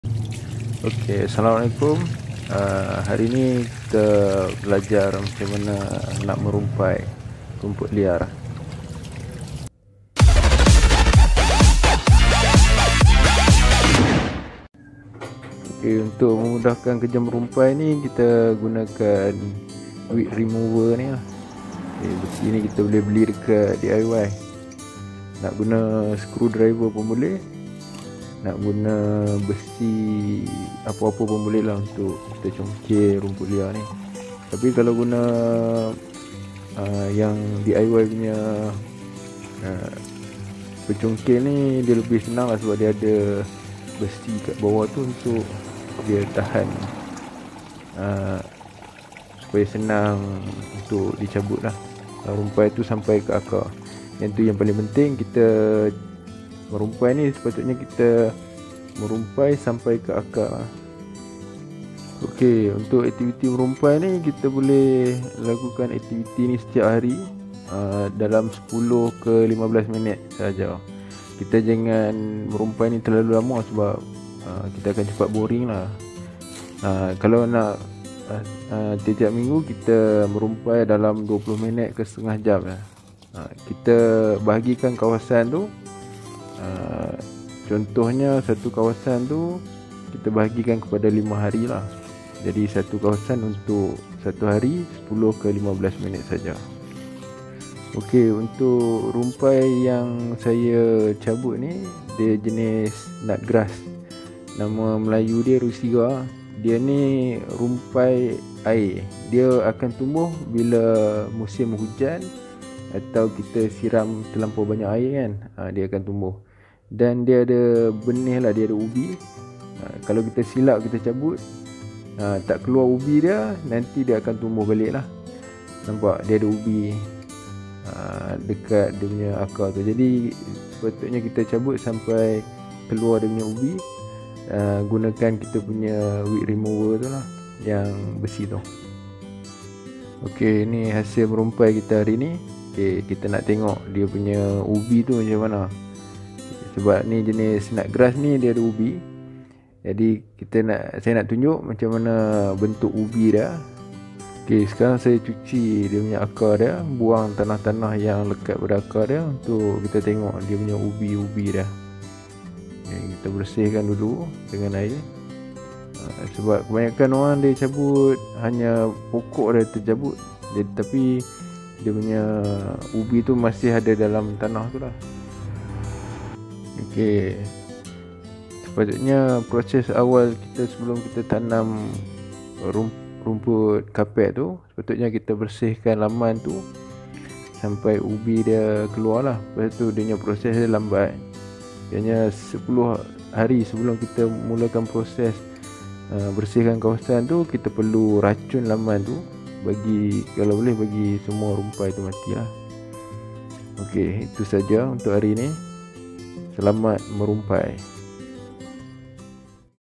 [18] Cara-cara cabut rumpai liar di laman karpet.
Merumpai merupakan salah satu aktiviti penjagaan laman rumput. Maka jadikan merumpai aktiviti bulanan atau harian.

Pencungkil rumpai ni boleh beli di bahagian kebun dekat Mr.Diy.

Get exclusive tips and tricks for your own lawn care!